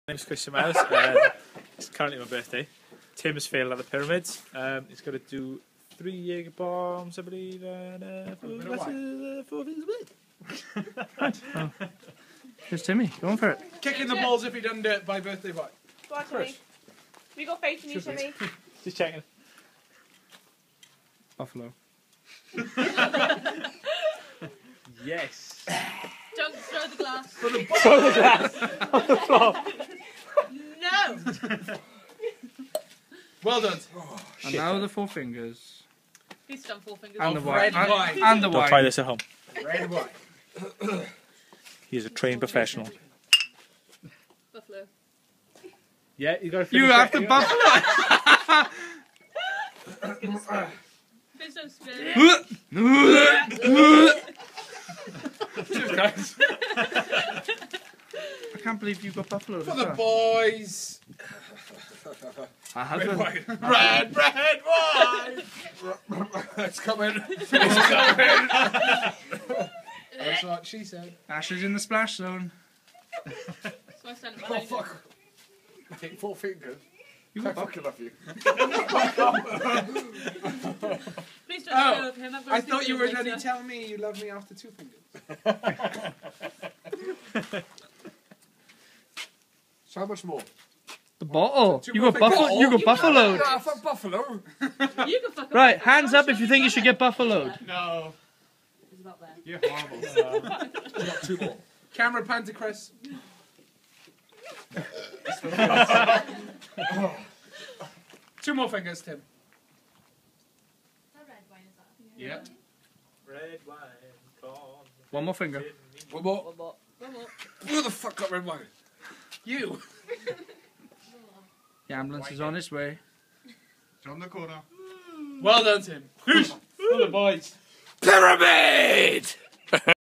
Drinking games - pyramid part 1
Tim lost at pyramid and here's his forfeit part 1.